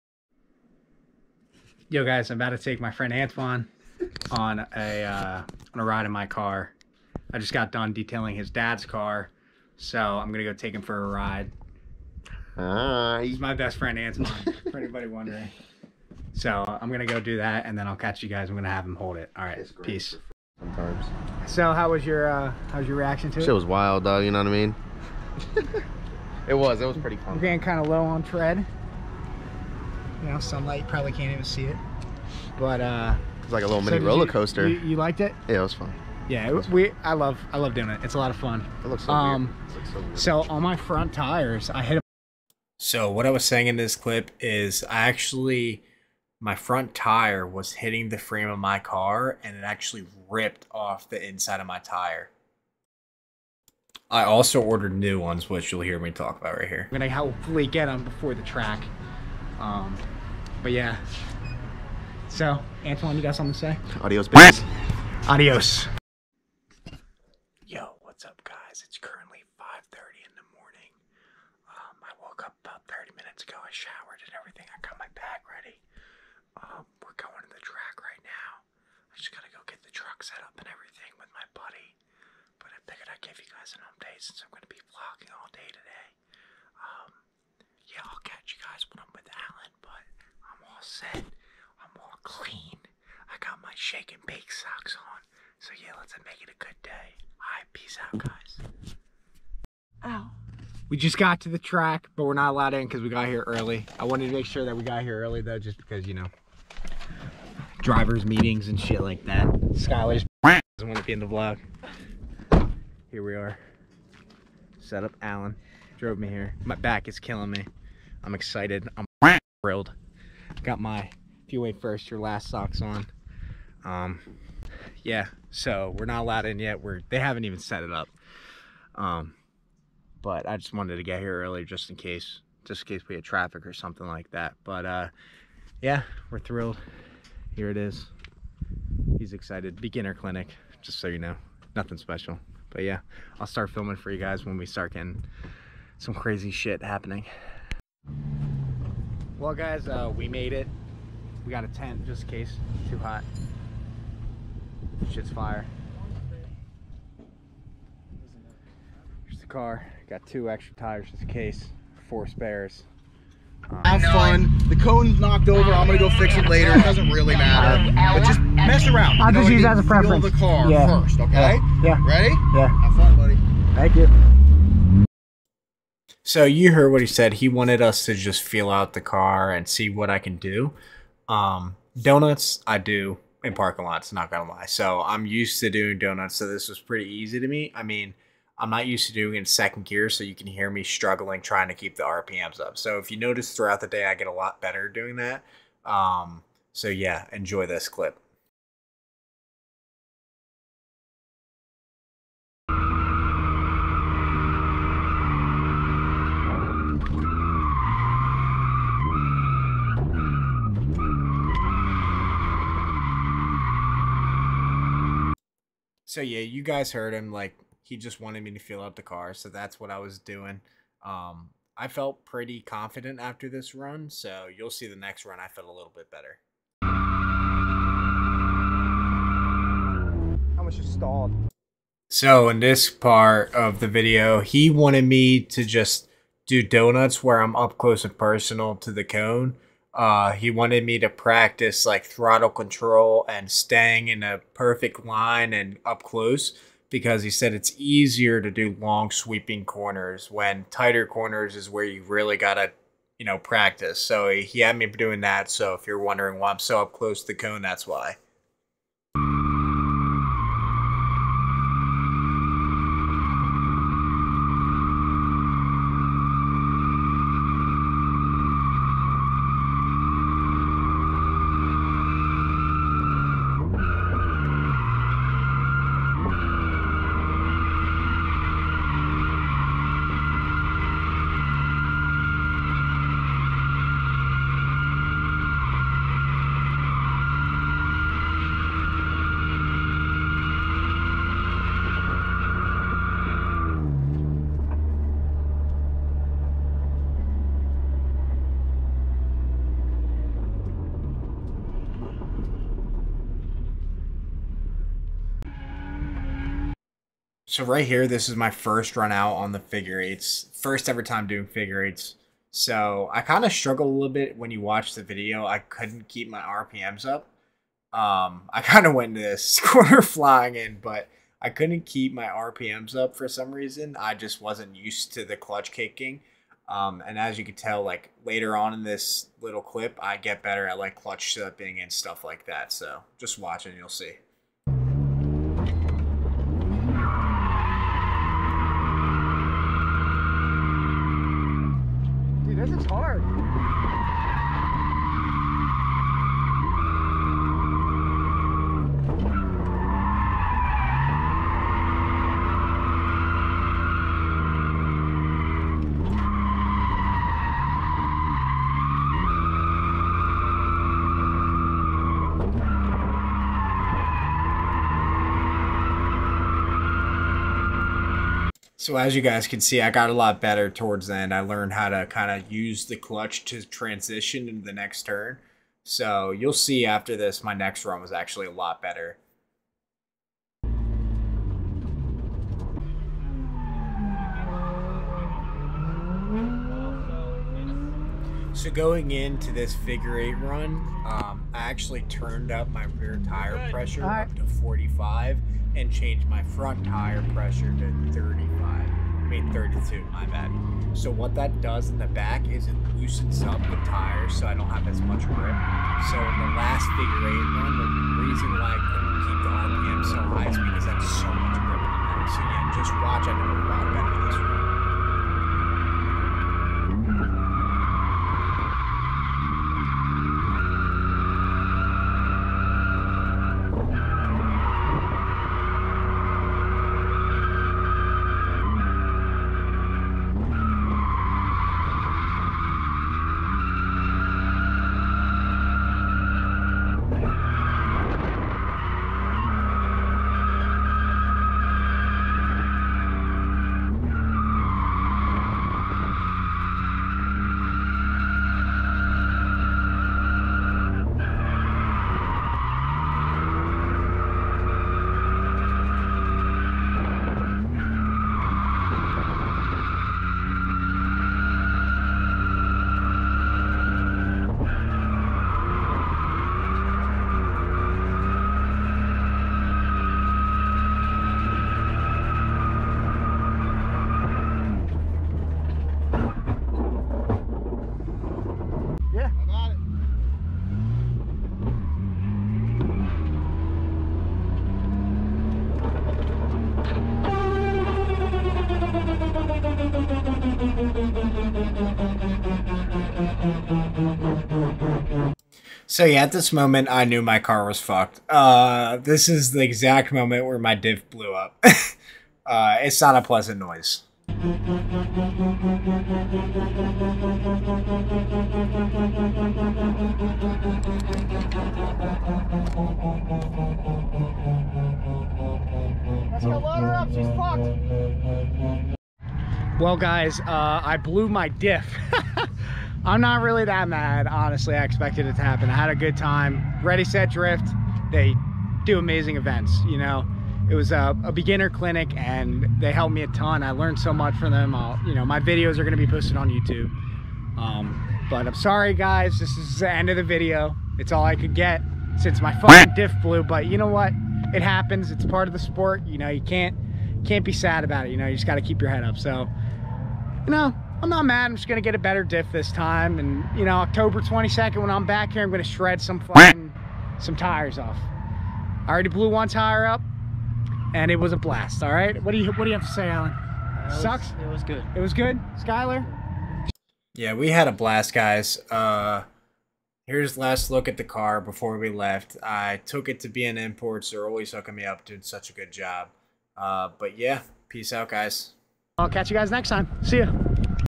My first drift event!!
wow! I had so much fun at the ready set drift event! they do such amazing work. I'm sorry about the glares and lighting in this video still learning everything on the camera lol! i felt so confident after this event with my drifting, also this was my first time drifting no expert here lol!! hope u guys enjoyed